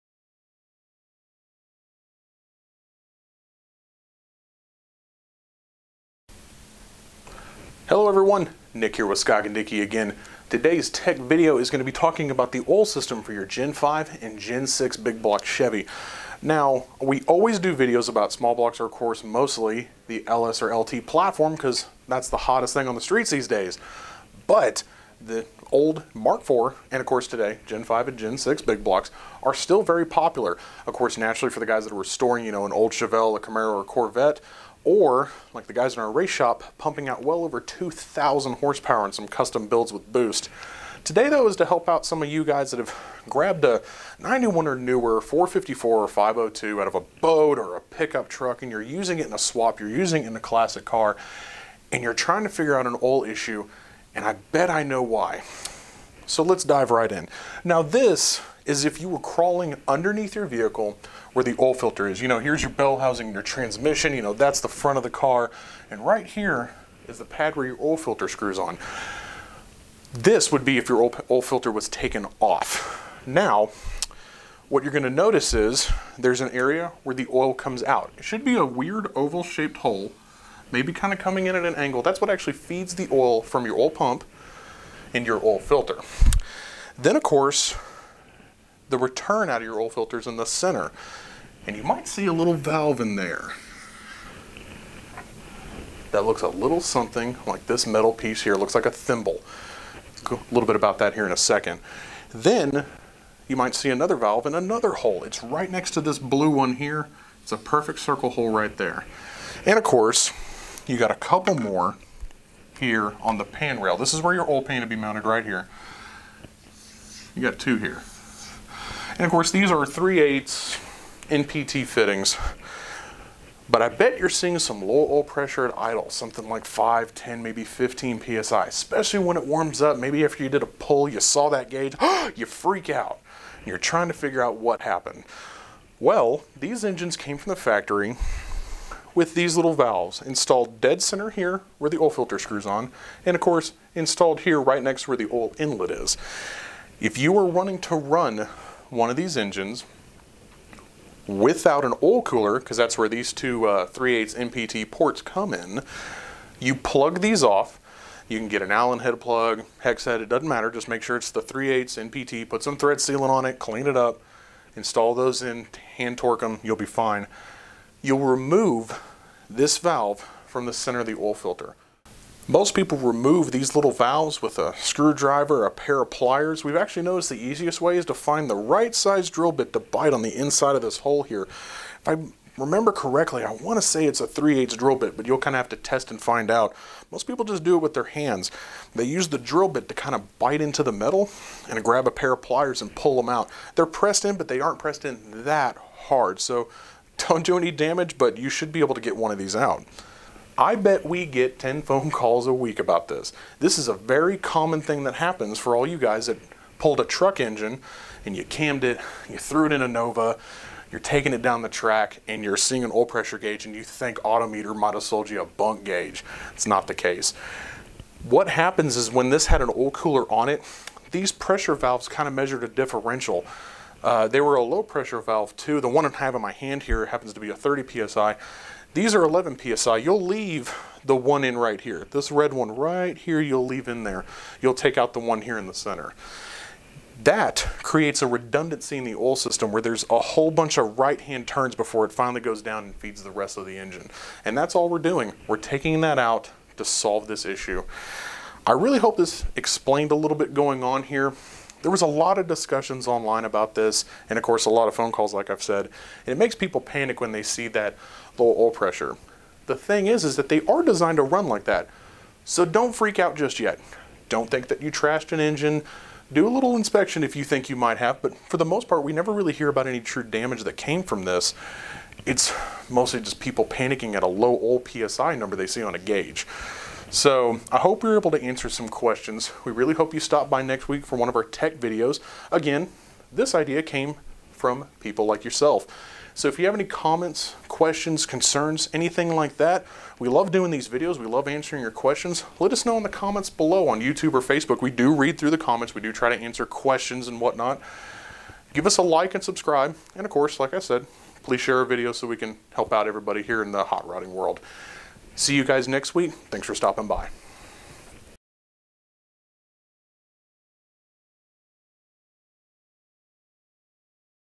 SDPC Tech Tips: Big Block Chevy Oil Filter Bypass
Nick goes over the Chevy big block with one important, albeit, tiny detail!

When using a Big Block from a truck, boat or other vehicle that had the plumbing in place for an oil cooler, some people might encounter a low oil pressure reading on their gauge.